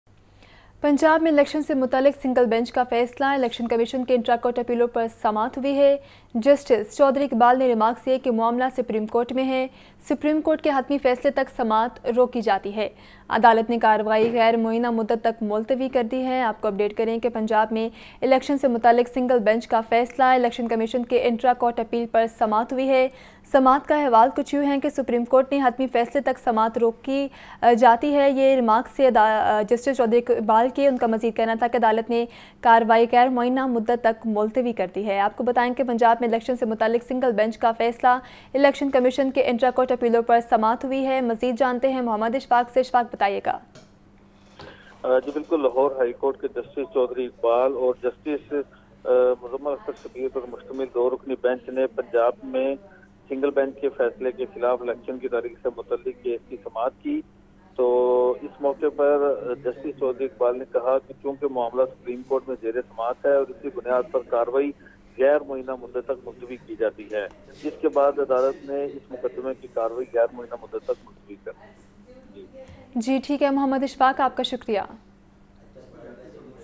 Breaking| Important News About Election Date From LHC | Lahore News HD
The Lahore High Court adjourned the hearing till February 27 on the appeal of the Governor and the Election Commission against the decision of the single bench to give the election date.

According to sources, a two-member bench of the Lahore High Court heard the appeal of the Governor of Punjab and the Election Commission against the decision of the single bench on the date of the election.

Advocate Azhar Siddique said that the position of the governor is constitutional but the governor of Punjab is running away from giving the date of the election. The position is constitutional, such words are not appropriate.

Justice Chaudhry Muhammad Iqbal inquired whether there is any provision in the constitution regarding the consultation of the Election Commission with the Governor to give the date of the election. It does not exist, I did not want to say this, but the President cannot give the date of the election like this.